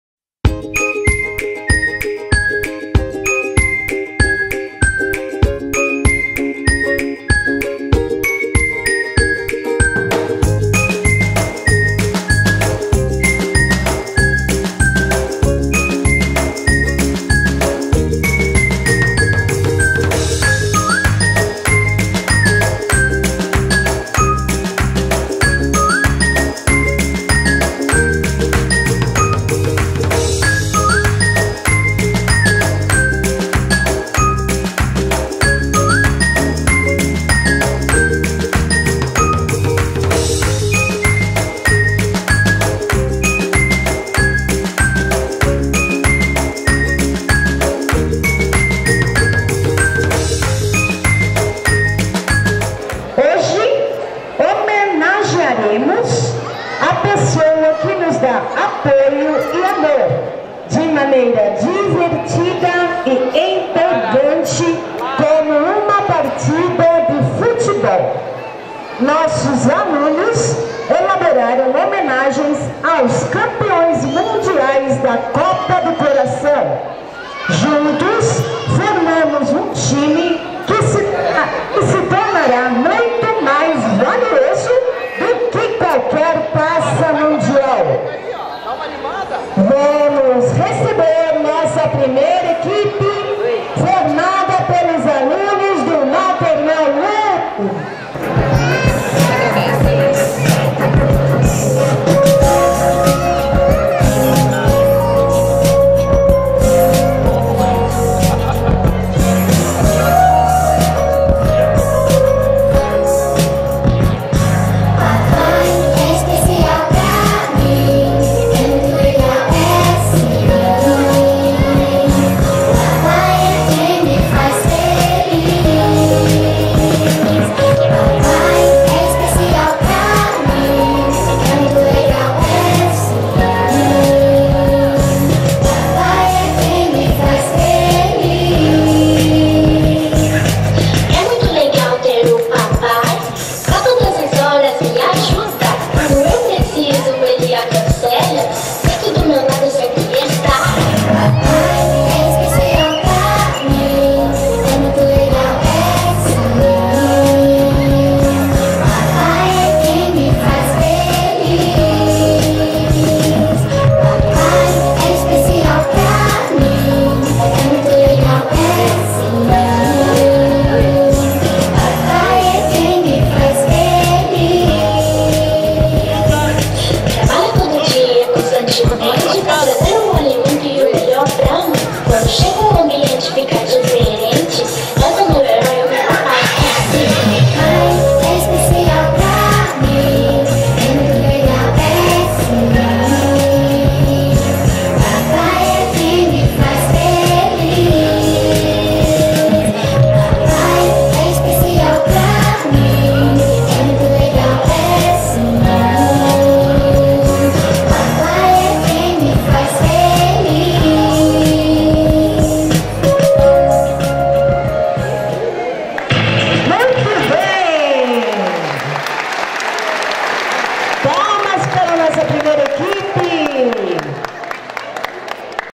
Dia dos Pais apresentação Maternal I
Apresentação aos pais dos alunos do maternal I do do Colégio Integral Bragança Paulista 2018